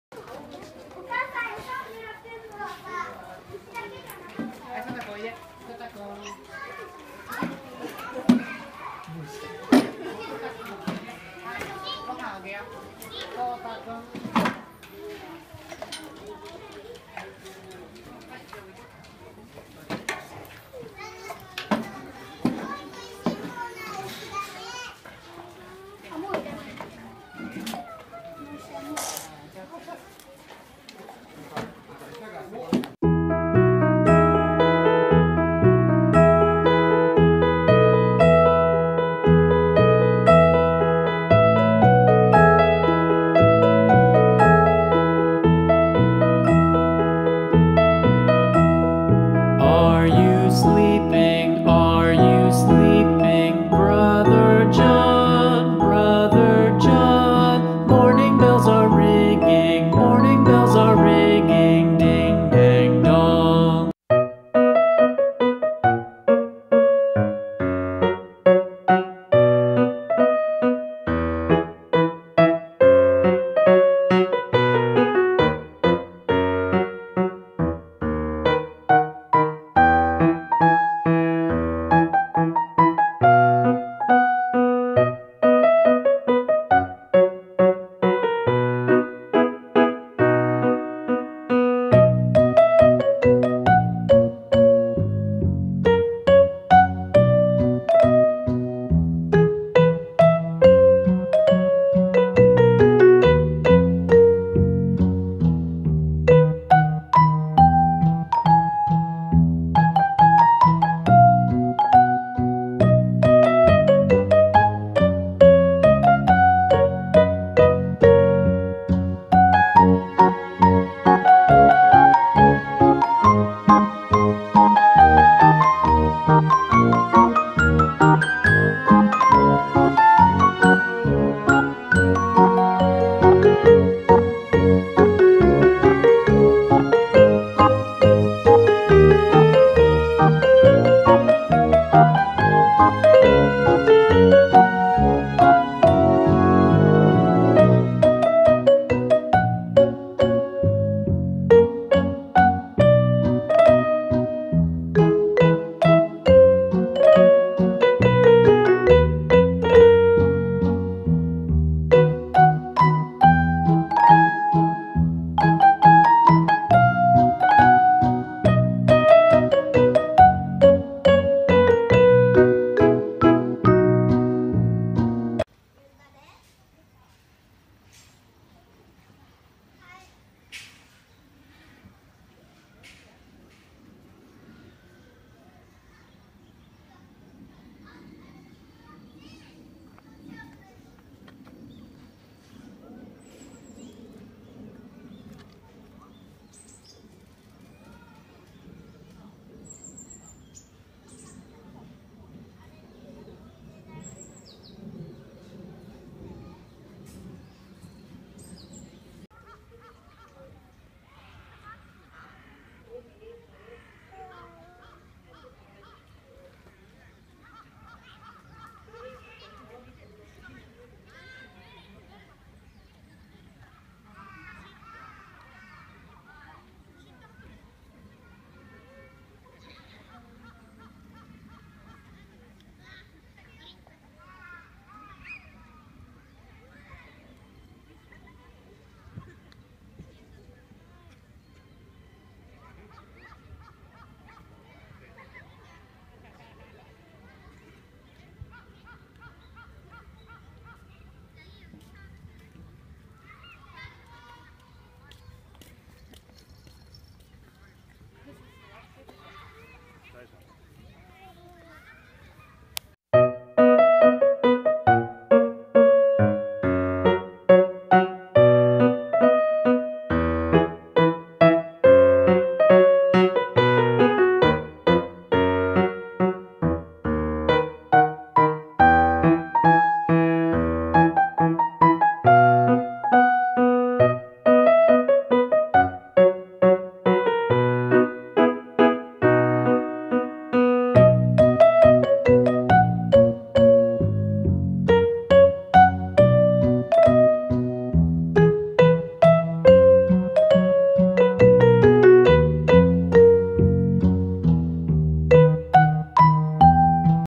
横浜こどもの国～牛や羊の餌やり、ポニー乗馬、芝生遊びなど
一日遊べる横浜「こどもの国」。
牛や羊の餌やり、ポニー乗馬、芝生遊びなどの様子です。